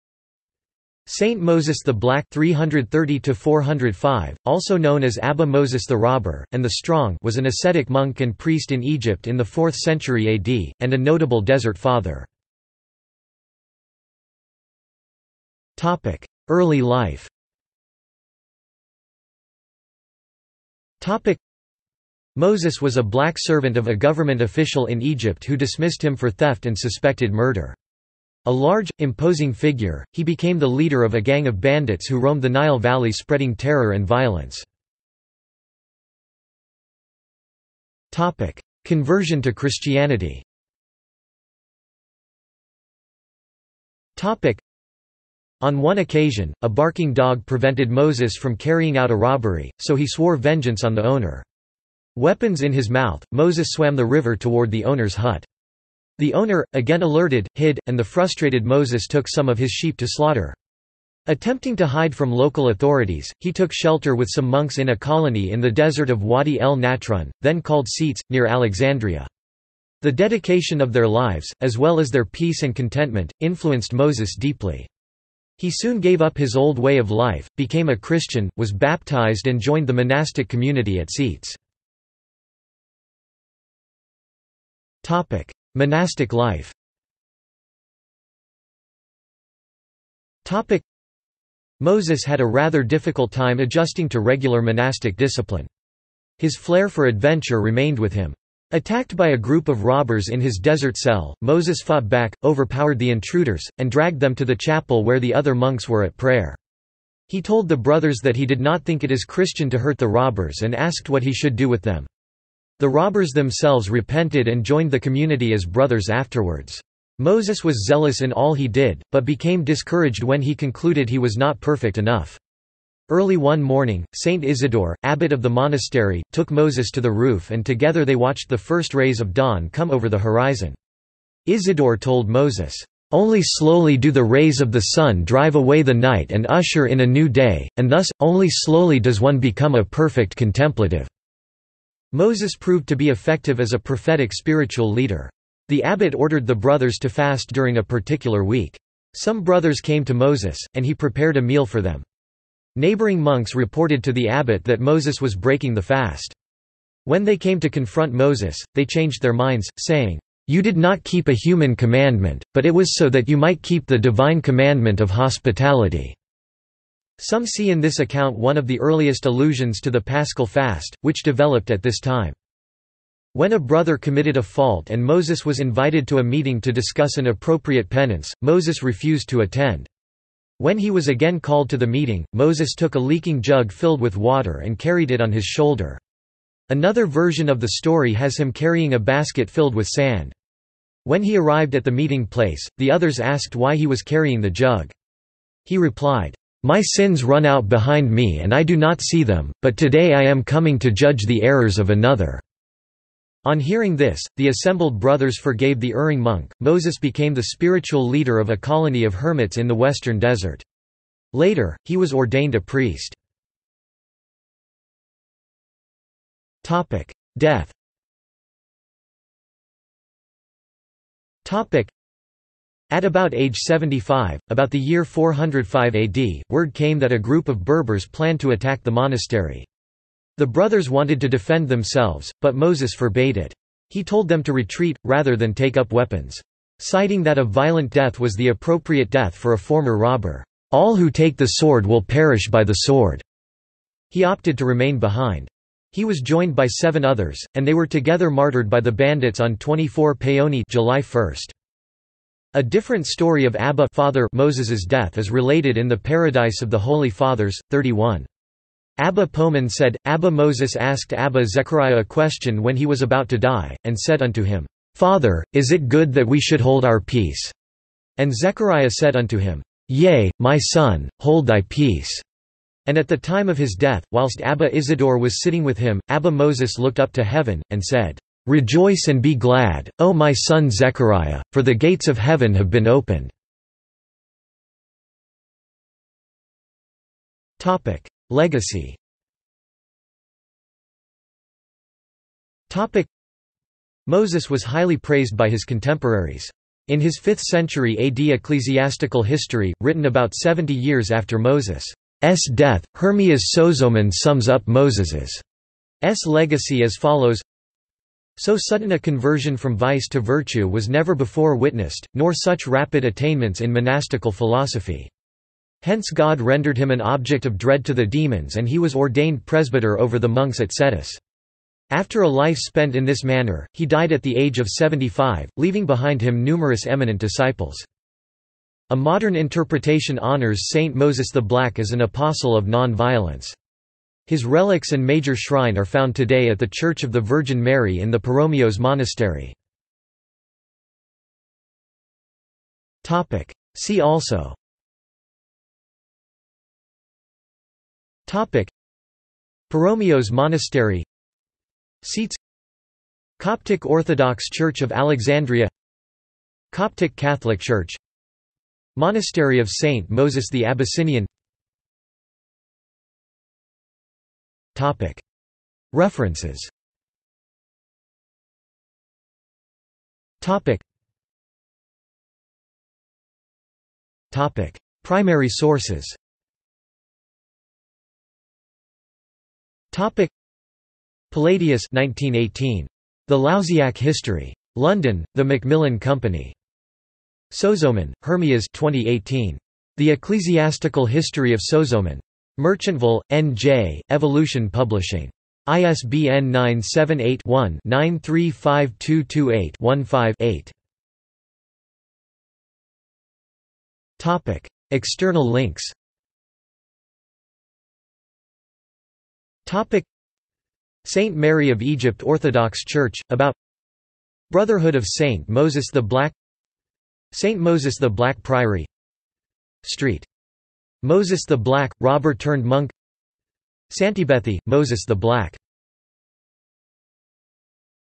Moses the Black | Wikipedia audio article
This is an audio version of the Wikipedia Article:
Moses the Black

SUMMARY
Saint Moses the Black (330–405), (also known as Abba Moses the Robber, and the Strong) was an ascetic monk and priest in Egypt in the fourth century AD, and a notable Desert Father.